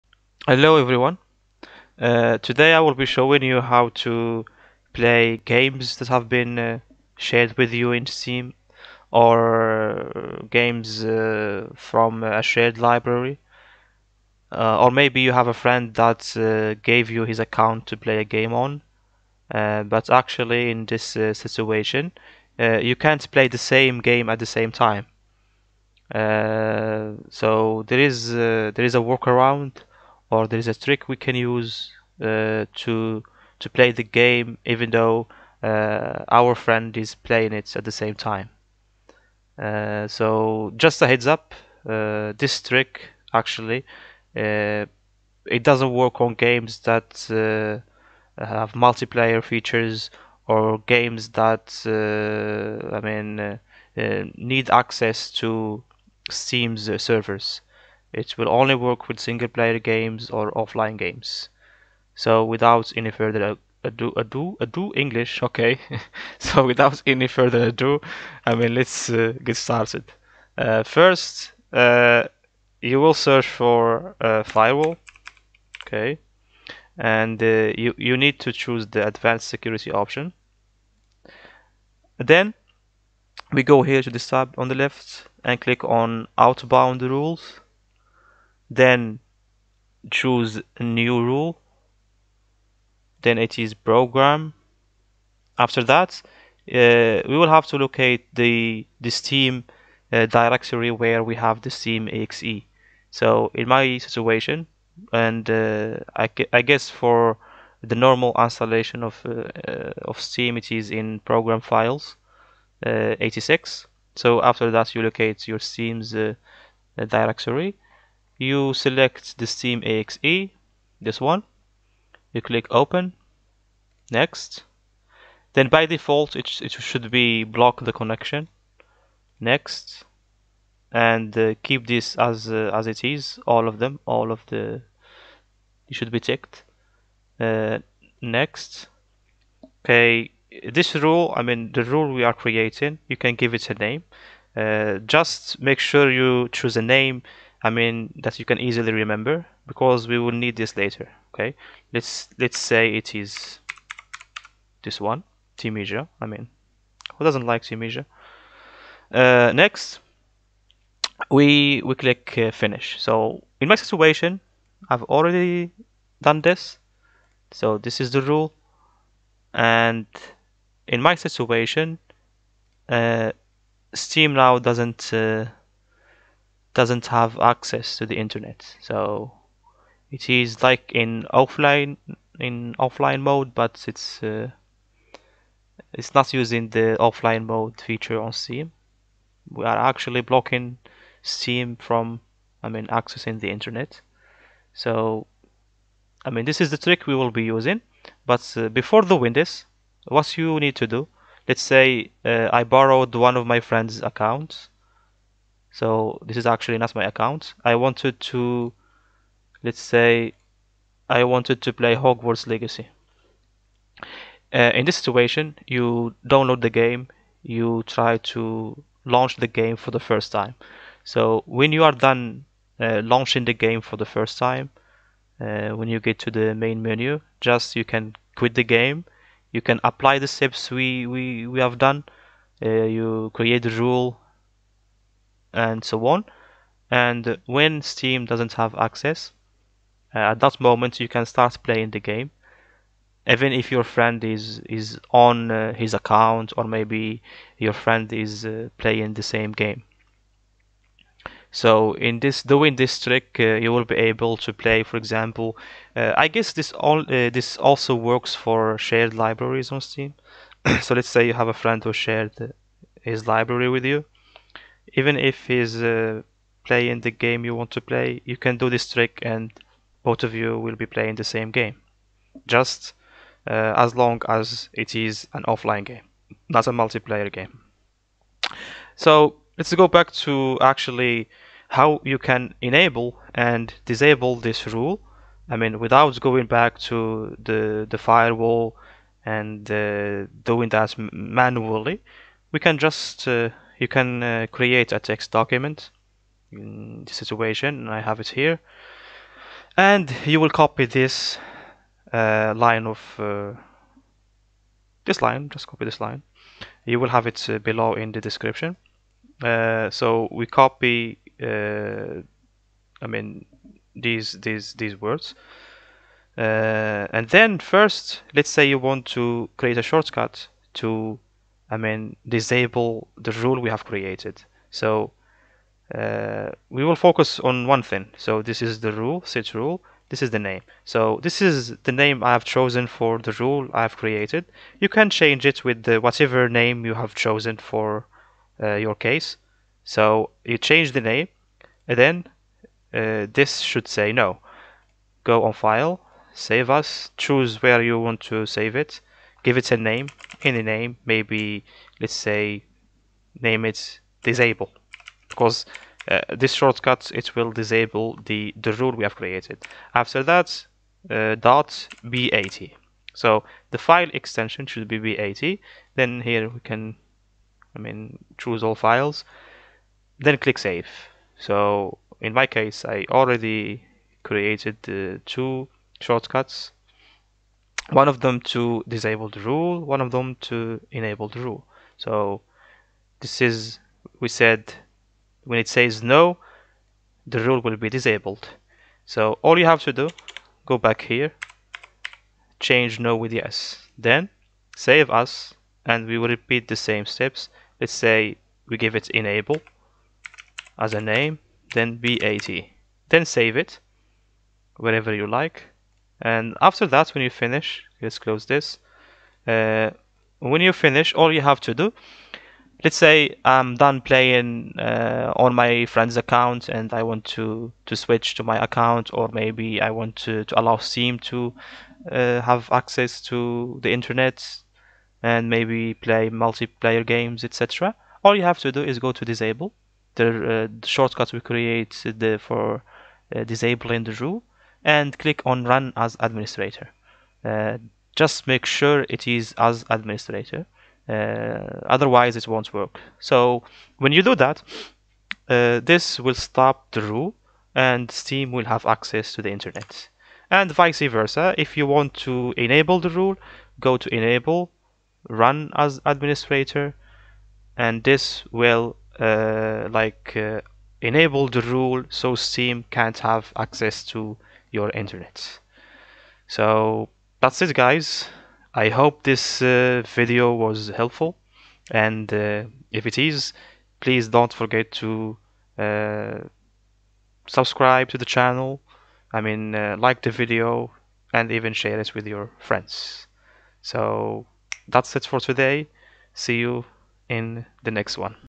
How to Play Steam Shared Games or Account While Owner's Using them
Here's a quick video on how to play steam games offline from a shared library or a friend's account on Windows. This trick will allow you to play games while your friend is online or playing the same game.

Like & sub you plebs :D

EnableRule.bat
netsh advfirewall firewall set rule name="timija" new enable=yes

DisableRule.bat
netsh advfirewall firewall set rule name="timija" new enable=no